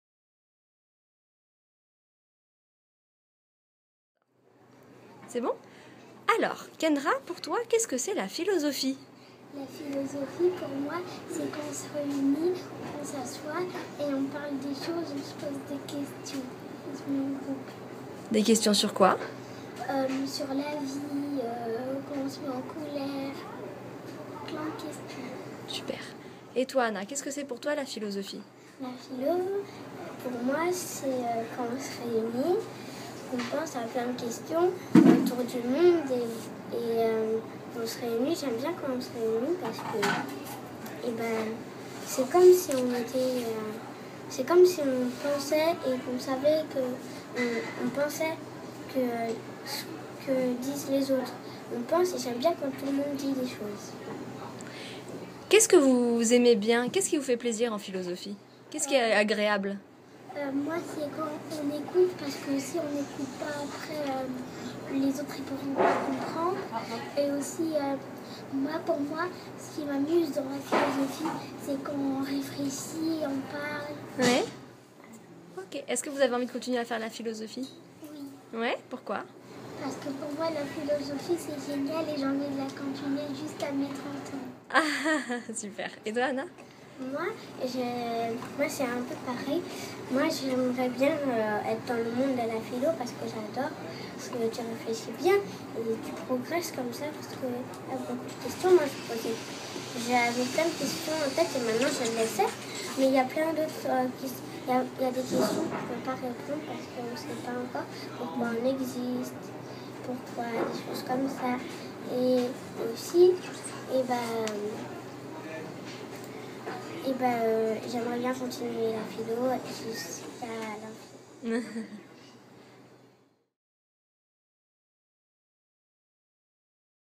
LaPhiloSelonKendrahEtAnna
A Romainville, on adore donner la parole aux jeunes penseurs ! Après Walid et Luka, voici comment Kendrah et Anna conçoivent la philosophie ! Elles s'expriment lors du bilan de leur année de philosophie : c'était en juin 2017 !